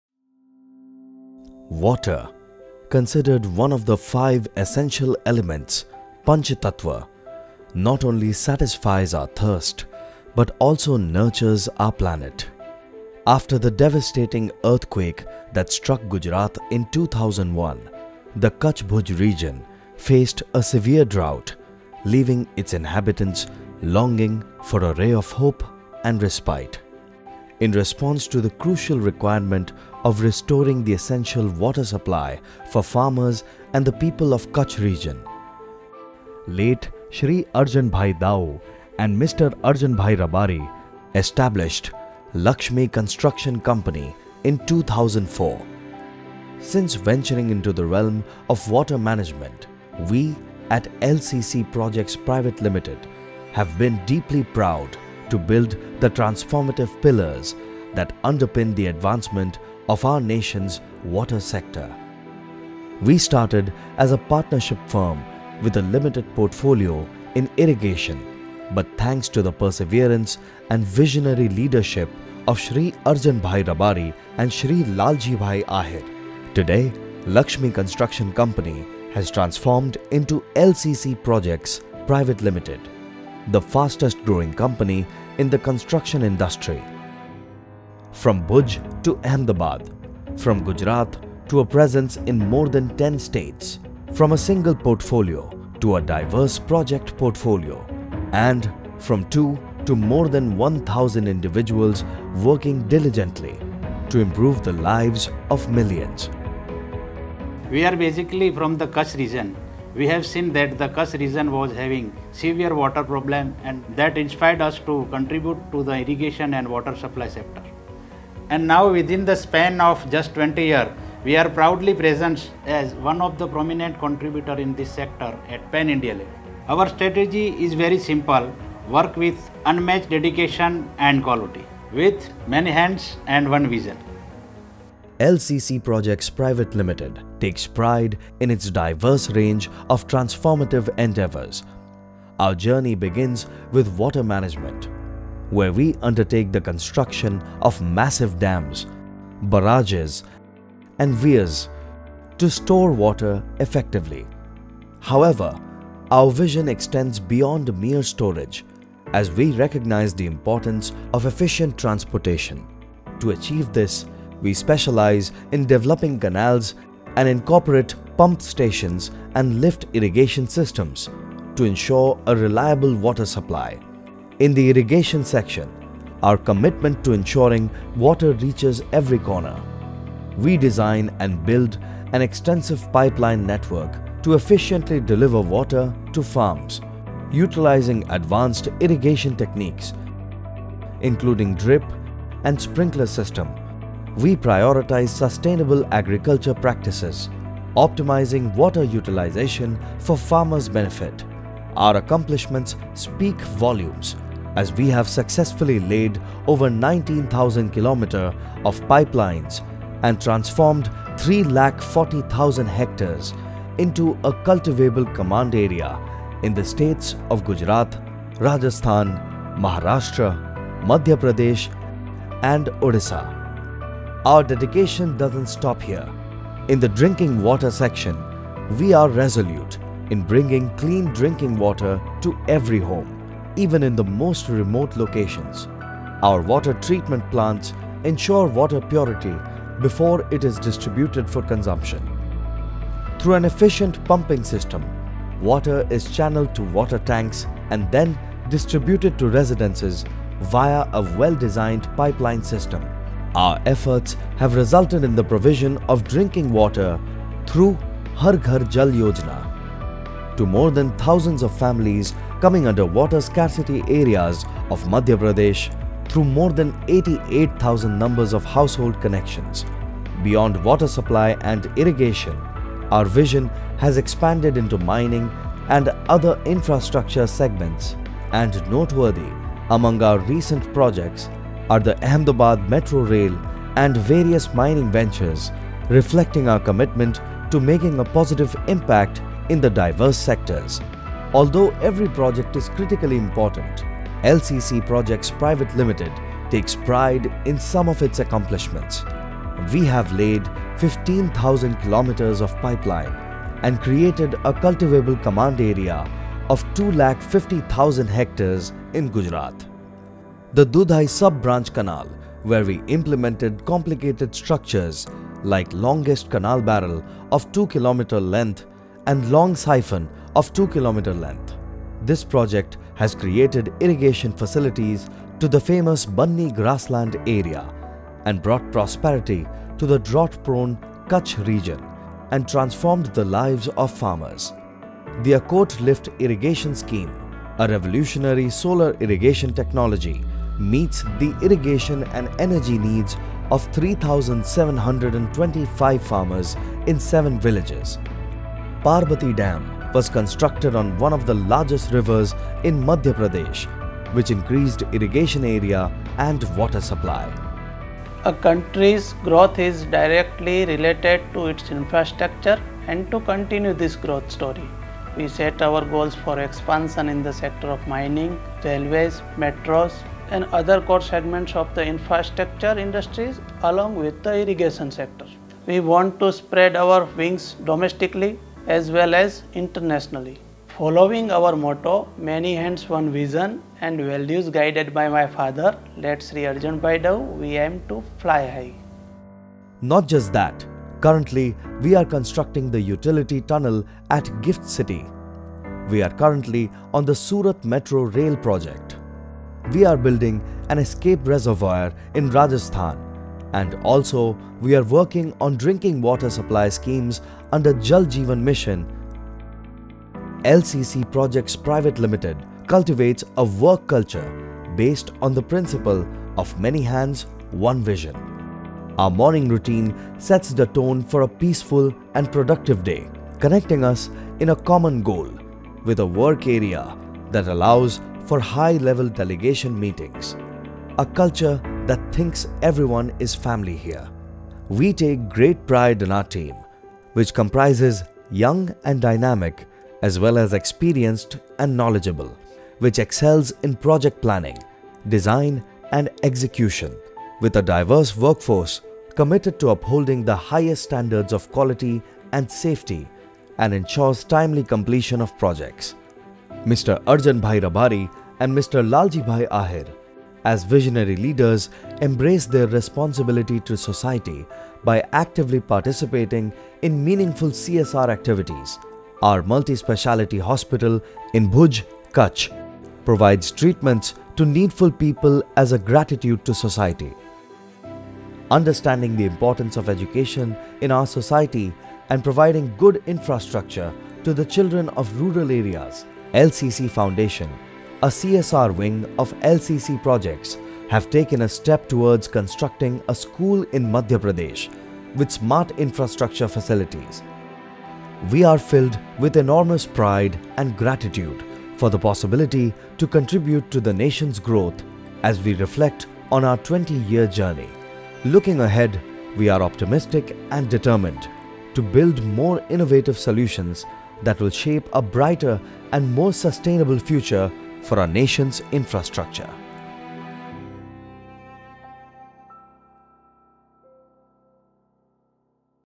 LCC | Corporate Documentary | Prayoga Films
Join us as we explore the dynamic history and evolution of LCC, a trailblazing corporation that has redefined industry standards. From its humble beginnings to its current stature as an industry leader, witness the relentless dedication, innovation, and vision that propelled LCC to the pinnacle of success.

Discover the key strategies and values that have been the driving force behind LCC's enduring success. Gain insights into their cutting-edge technologies, sustainable practices, and commitment to global excellence. This documentary sheds light on the people, ideas, and initiatives that continue to shape the legacy of LCC.